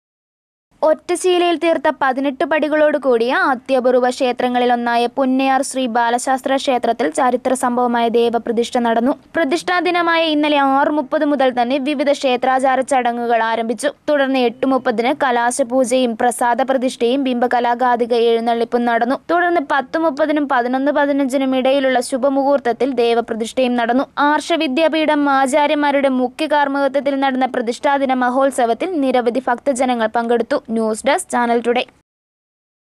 ഒറ്റ ശിലയില്‍ തീര്‍ത്ത പതിനെട്ട'് പടികളുള്ള അത്യാപൂര്‍വ ക്ഷേത്രം
ഒറ്റ ശിലയില്‍ തീര്‍ത്ത പതിനെട്ട'് പടികളുള്ള അത്യാപൂര്‍വ ക്ഷേത്രങ്ങളിലൊന്നായ പുന്നയാര്‍ ശ്രീ ബാലശാസ്ത ക്ഷേത്രത്തില്‍ ദേവ പ്രതിഷ്ട നടന്നു. പ്രതിഷ്ടാദിന മഹോത്സവത്തില്‍ നിരവധി ഭക്തരാണ് പങ്കെടുത്തത